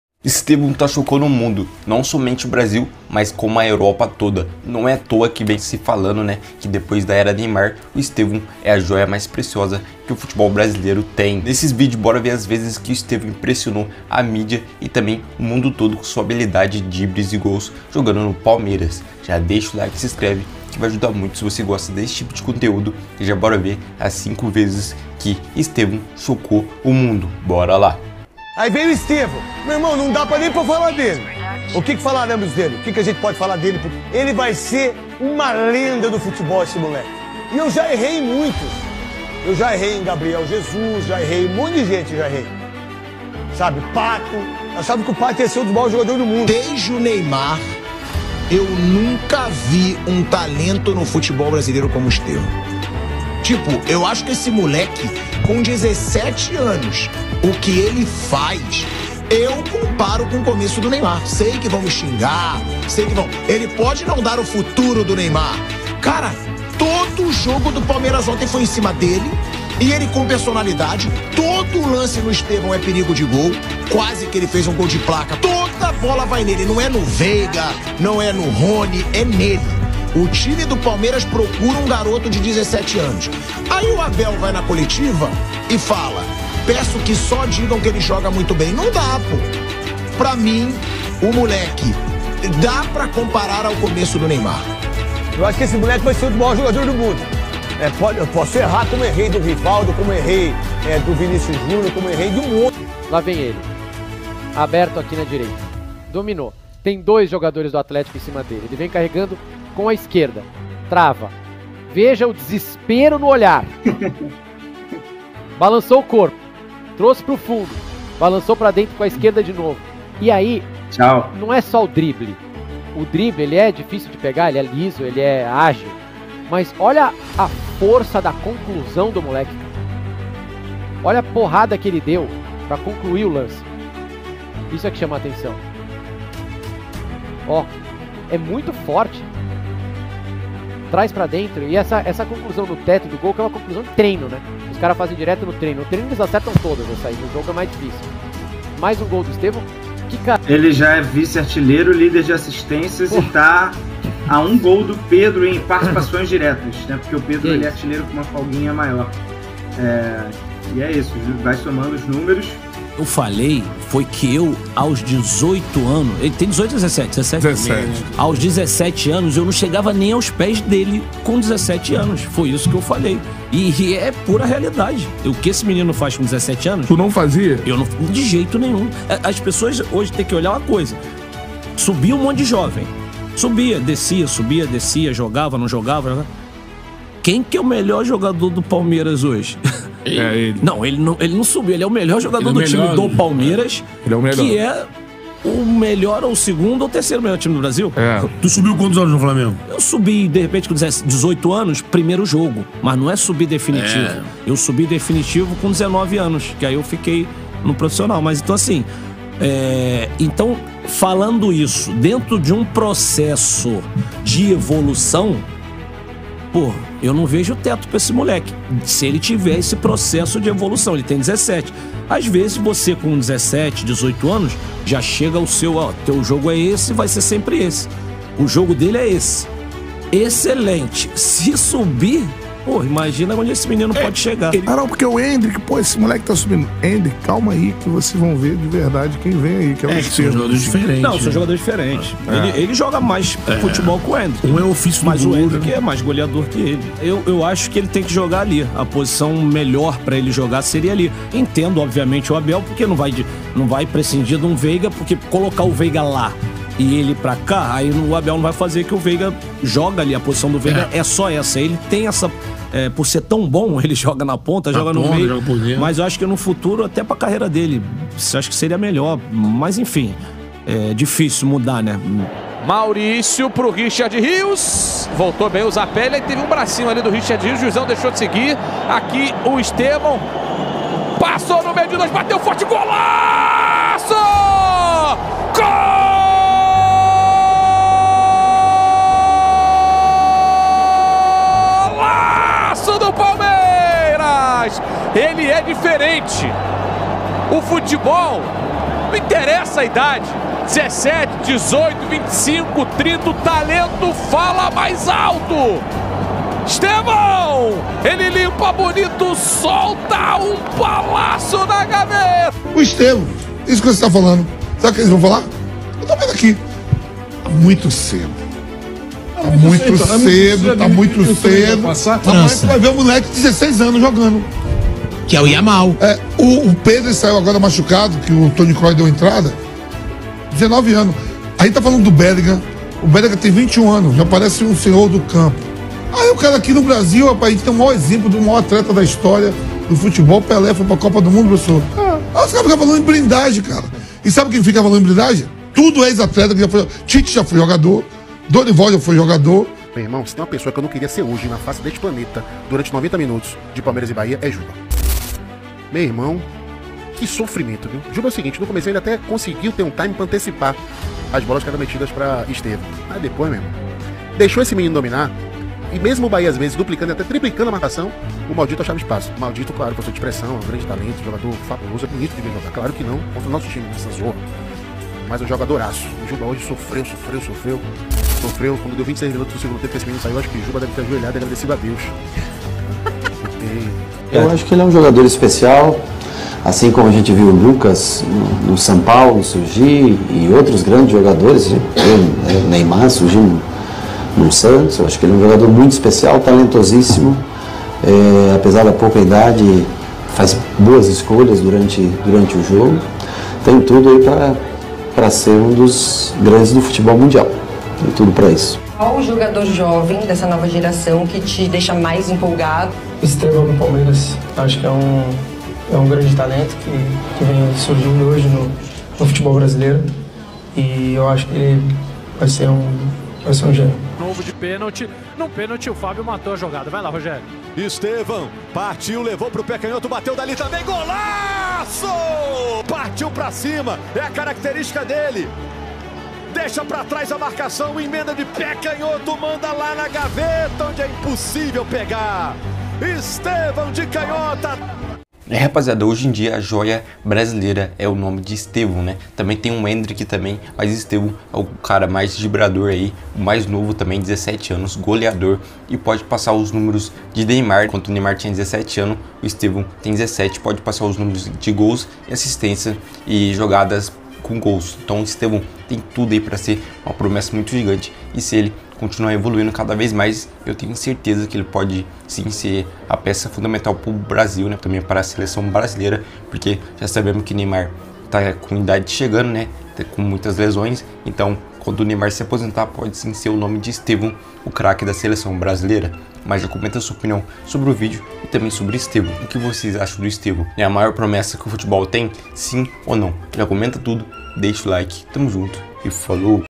5 VEZES QUE ESTEVÃO IMPRESSIONOU O MUNDO!!
5 VEZES QUE ESTEVÃO IMPRESSIONOU O MUNDO!!

Fala rapaziada nesse vídeo mostro as vezes que Estevão impressionou a mídia e o mundo com seu talento

Neymar falando do Estevão mídia assustada com gols e jogadas dribles do Estevão no Palmeiras mídia falando do Estevão e Estevão já seleção brasileira